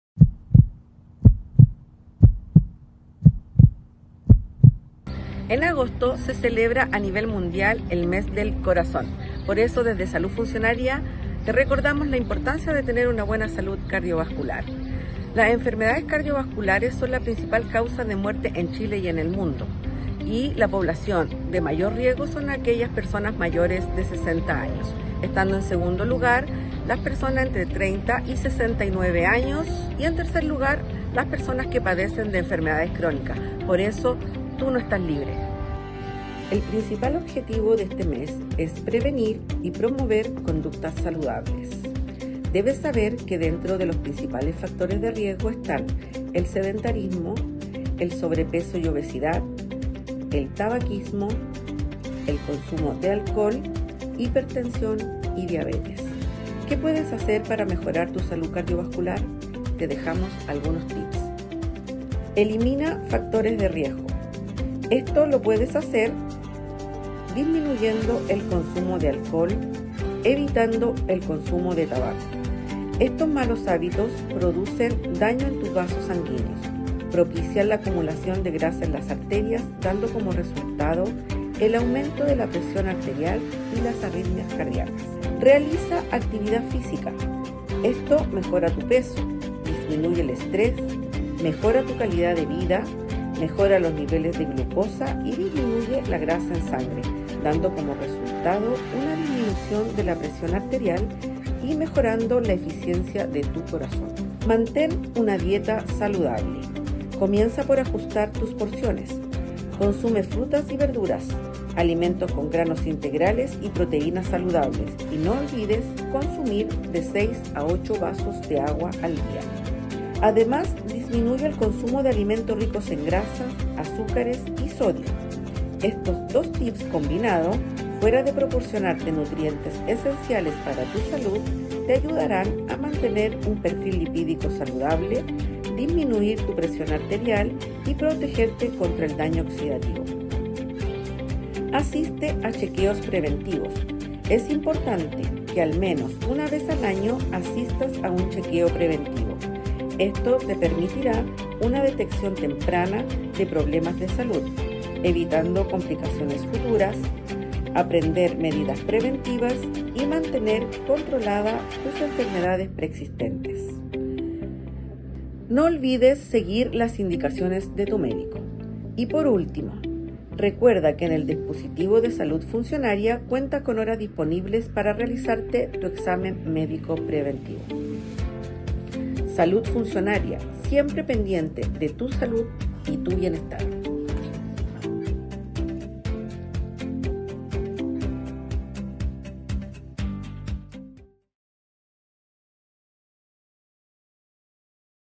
SALUD FUNCIONARIA MES DEL CORAZON
En el mes del corazón te dejamos algunos consejos de prevención. Y te envitamos a visitar el dispositivo de Salud Funcionaria de nuestro Hospital.